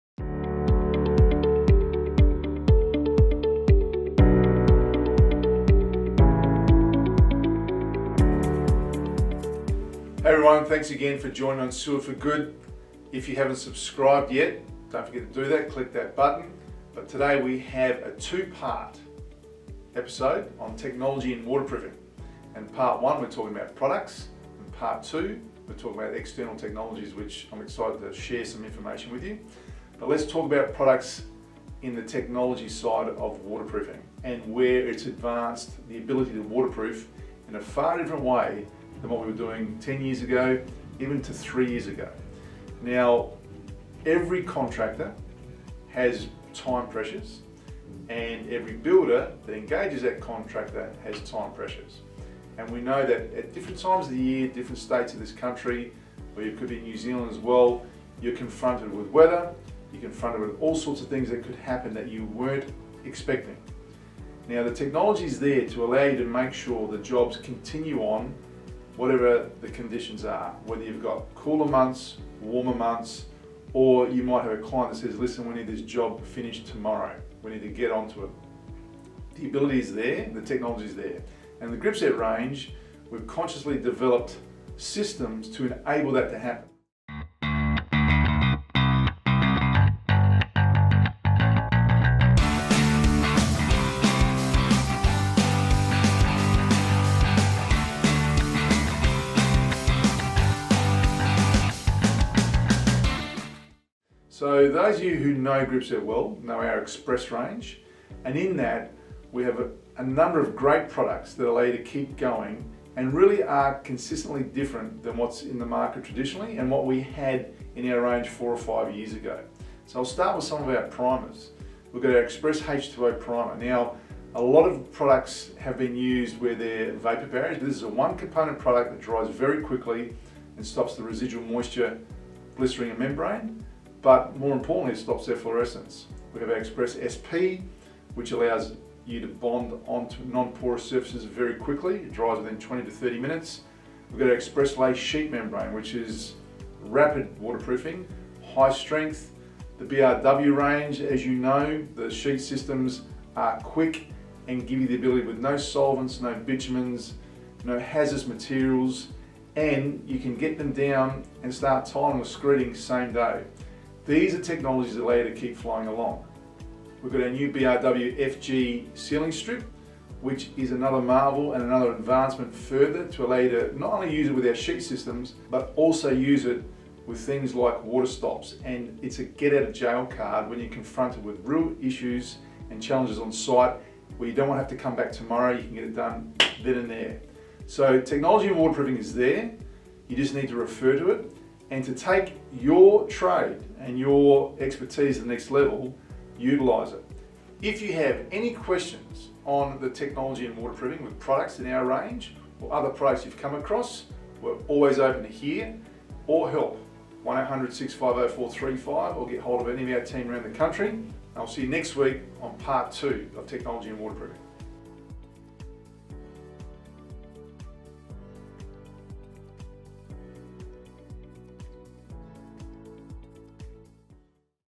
Technology in Waterproofing Part 1
On this episode of Sealed For Good we discuss Technology in Waterproofing Products and why you should be taking advantage of this.